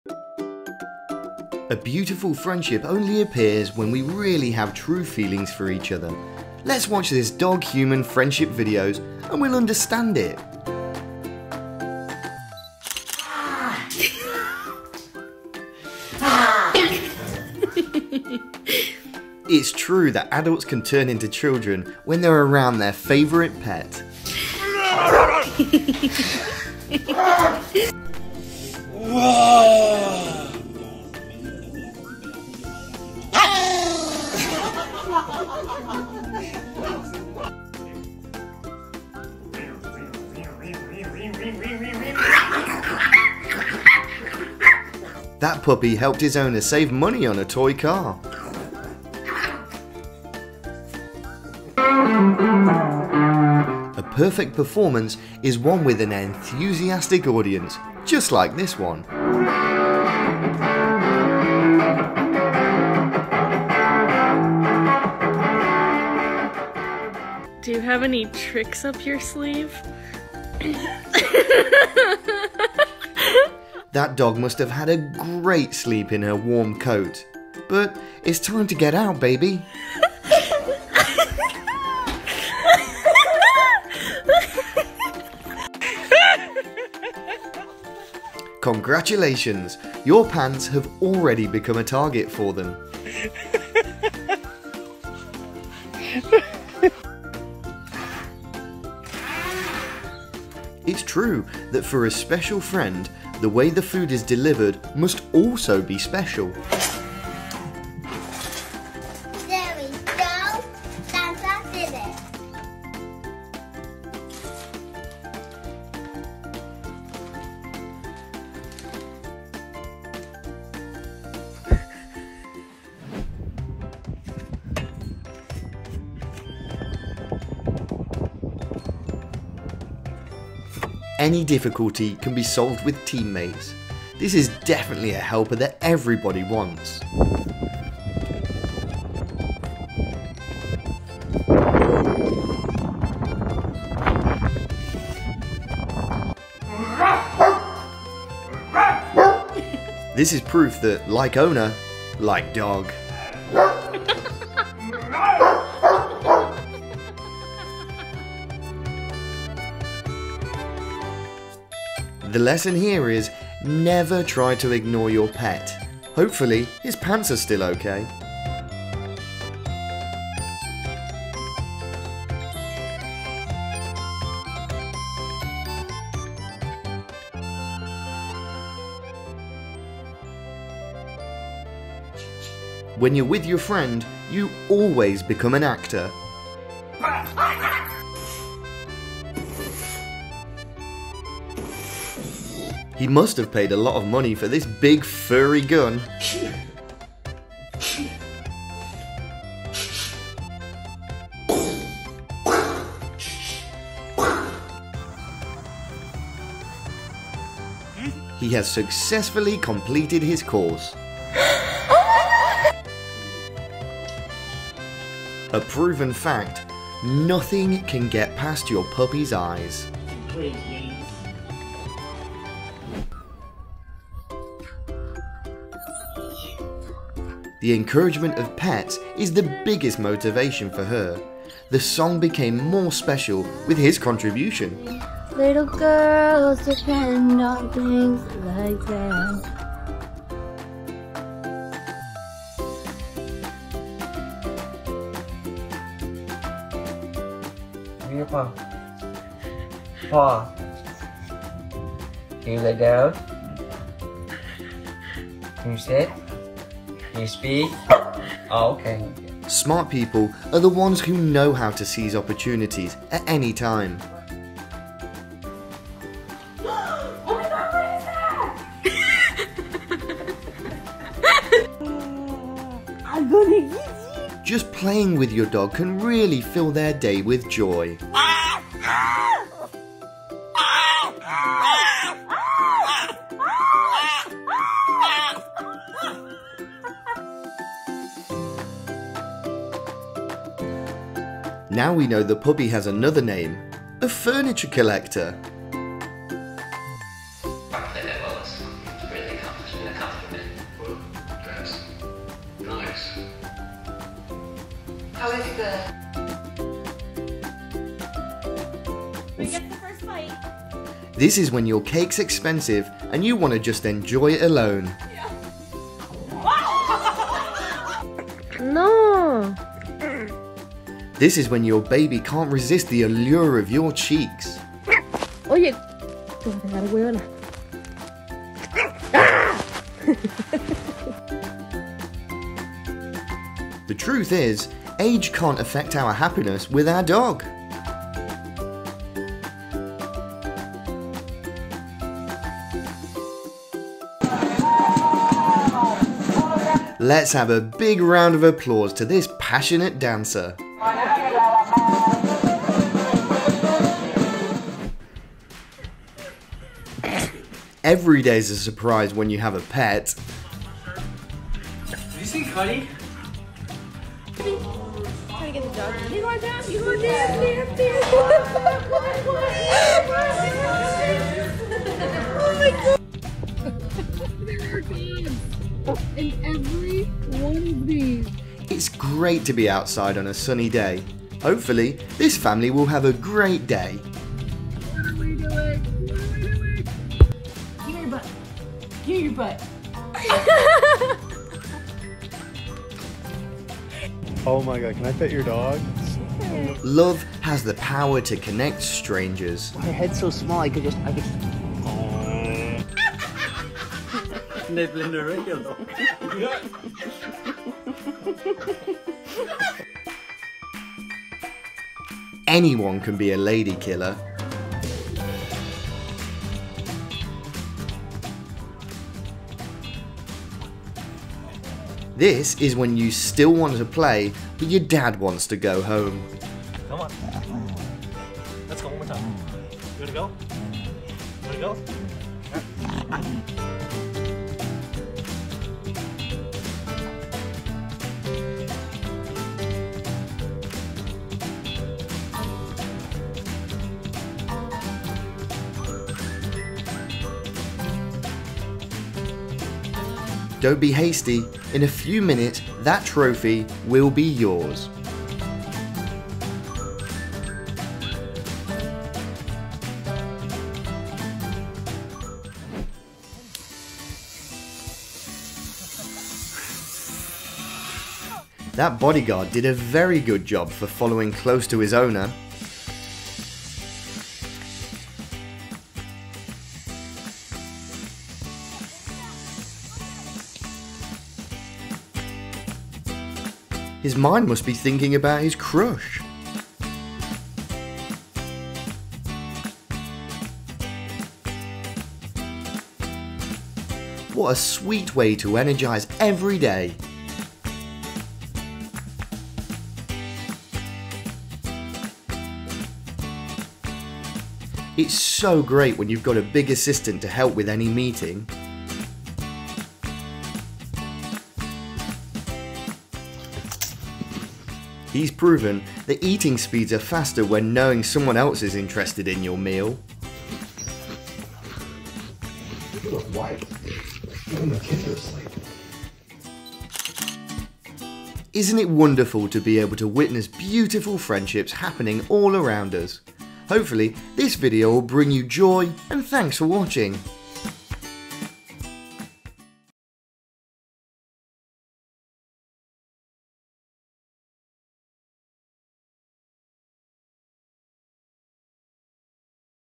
Having A Pet Is So Fun! Funniest Dog and Their Human are best friends
A beautiful friendship only appears when we really have true feelings for each other. Let's watch this dog-human friendship video to understand it.
Hope you guys have a good time on our channel!

Funny Cat and Their Human will make you EXPLODE LAUGHING - Cute Cat Show Love